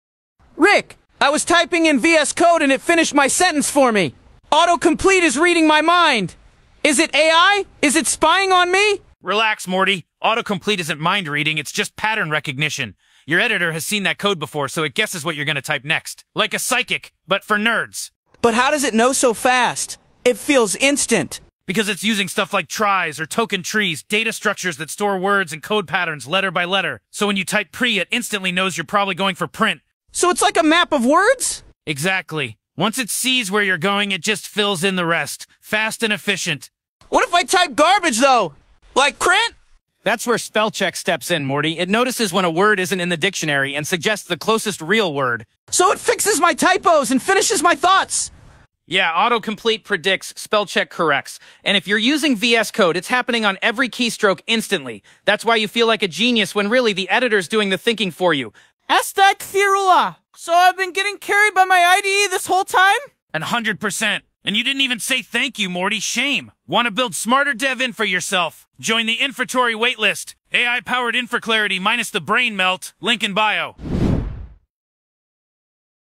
⚡ Rick Explains Autocomplete: How Your IDE Reads Your Mind
⚡ Join the Infratory waitlist – AI-powered infra clarity without the brain melt.

Morty’s freaking out because VS Code is finishing his thoughts.
Rick explains: it’s not magic — it’s tries, token trees, and real-time code prediction.

💡 Covered in under 85 seconds:
• Autocomplete in action (VS Code)
• Tries & token trees = speed
• Spellcheck vs Autocomplete
• Why your code editor deserves a “thank you”

👉 Want to build smarter dev infra yourself? Join the Infratory waitlist.